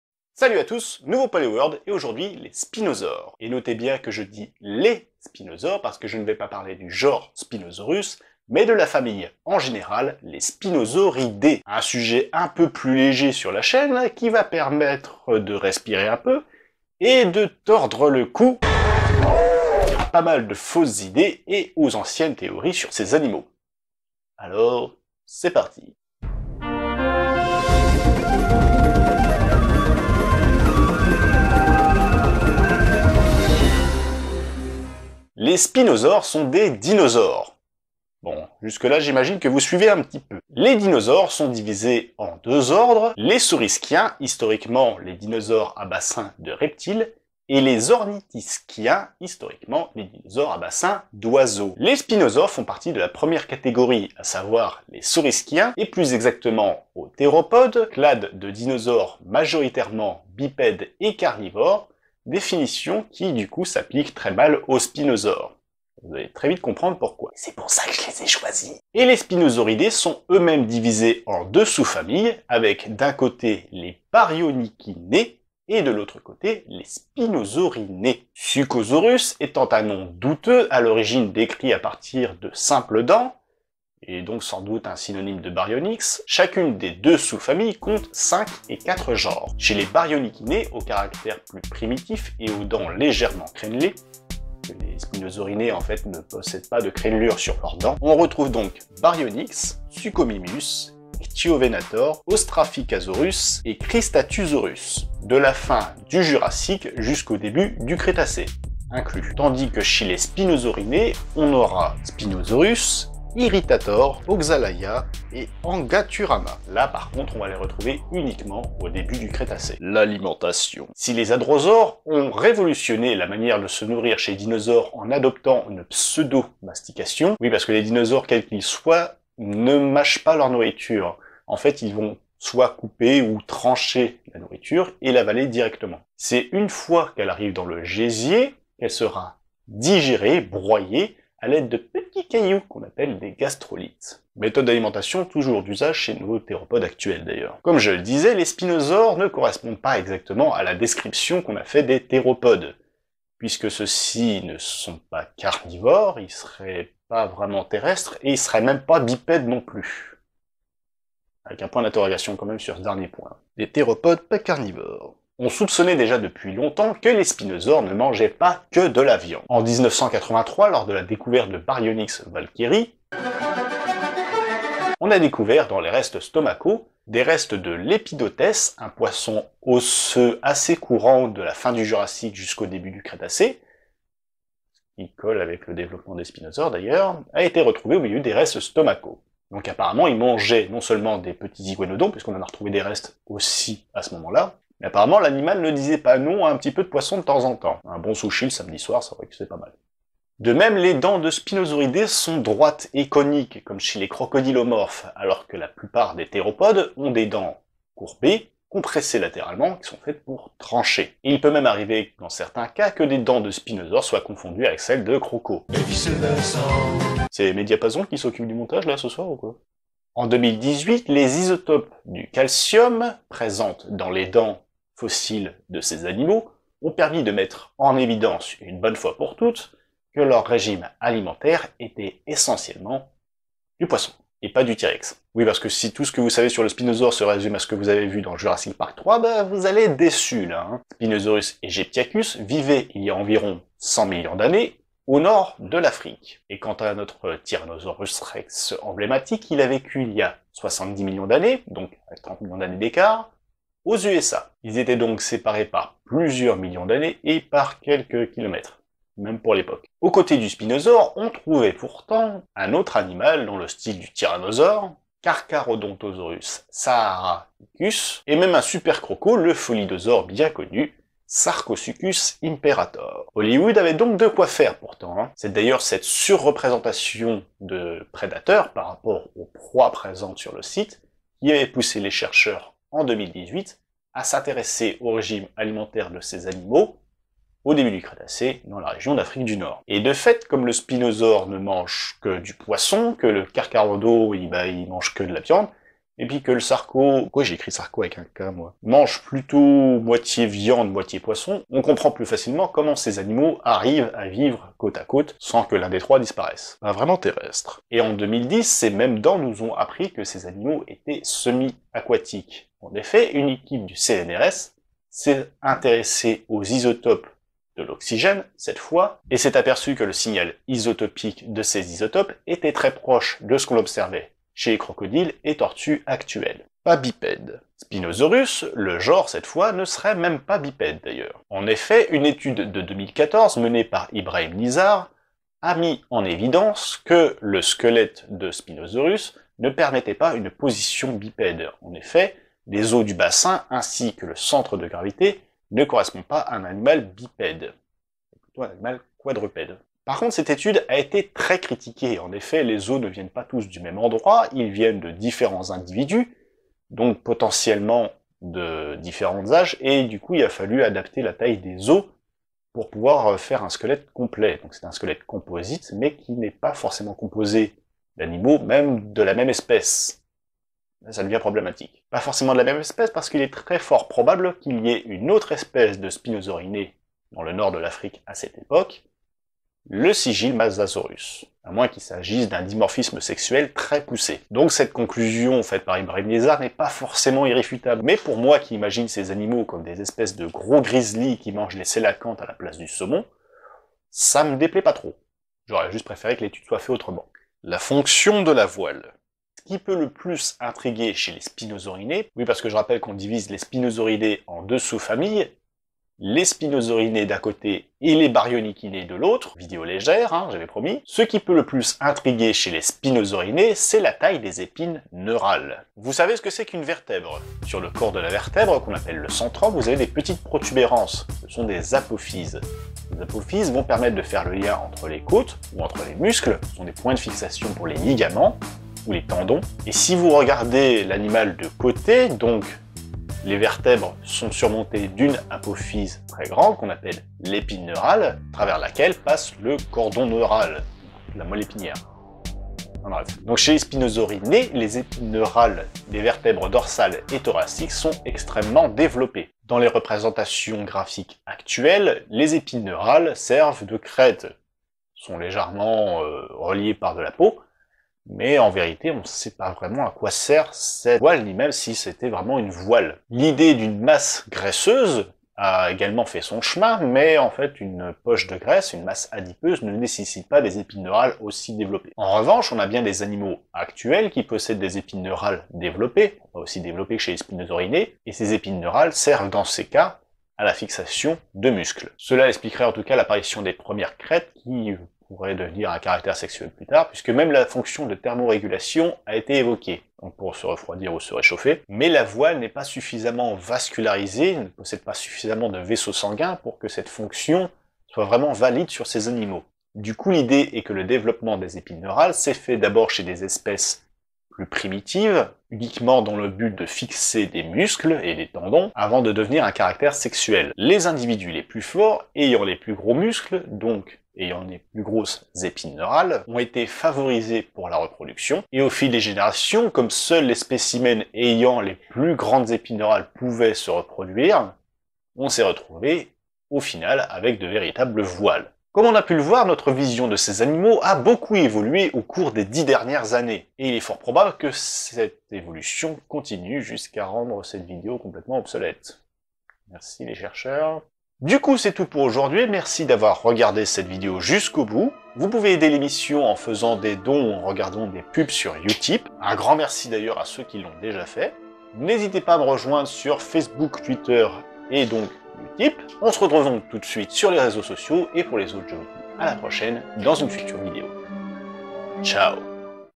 PALÉOWORLD 30 - LES SPINOSAURES
► Mode de vie semi aquatique ; Amiot, R., Buffetaut, E., Lécuyer, C., Wang, X., Boudad, L., Ding, Z., ... & Mo, J. (2010). Oxygen isotope evidence for semi-aquatic habits among spinosaurid theropods. Geology, 38(2), 139-142.

►  Une majorité de spinosauridés pourrait être exclusivement piscivore et la moitié d'entre eux étaient au moins partiellement piscivore ; Hassler, A., Martin, J. E., Amiot, R., Tacail, T., Godet, F. A., Allain, R., & Balter, V. (2018). Calcium isotopes offer clues on resource partitioning among Cretaceous predatory dinosaurs. Proceedings of the Royal Society B: Biological Sciences, 285(1876), 20180197.

► Spinosaurus quadrupède ; Ibrahim, N., PC Sereno, C. Dal Sasso, S. Maganuco, M. Fabbri, DM, DM,… Iurino, DA (2014). Adaptations semi-aquatiques dans un dinosaure prédateur géant. Science, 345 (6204), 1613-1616. doi: 10.1126 / science.1258750

Bonjour et bienvenu !

Vous êtes vous déjà demandé ce qu'étaient les spinosaures ? ... Non ? oO ... Bon tans pis...